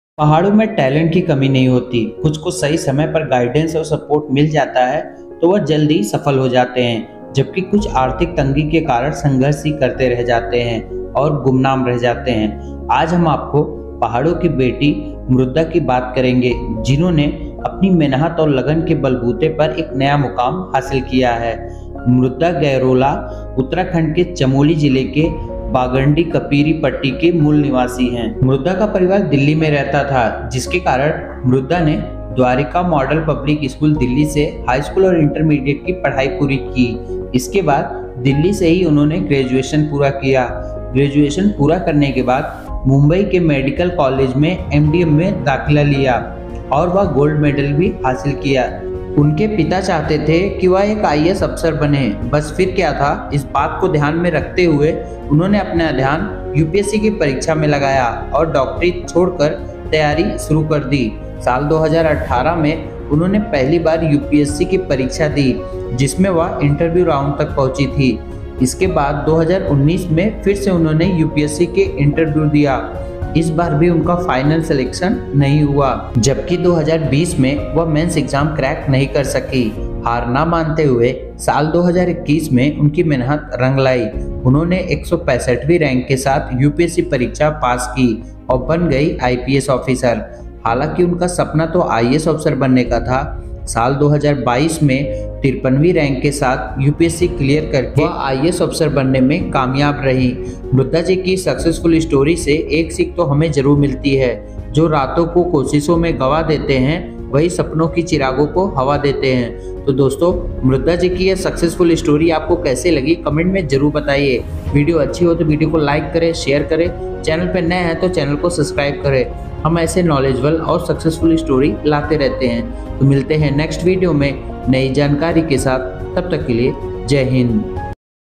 IAS officer successful mudra story@ias motivation story
IAS officer successful mudra story#upscrank54

          दोस्तों आज की इस वीडियो मैं हम आपको मुद्रा आईएएस ऑफिसर के बारे में बताएंगे कैसे इन्हें तीन बार असफलता मिलने के बाद भी हार नहीं मानी और चौथी बार प्रयास करके अपने सपने को साकार किया तो चलिए लिए जानते हैं आईएएस ऑफिसर मुद्रा के बारे में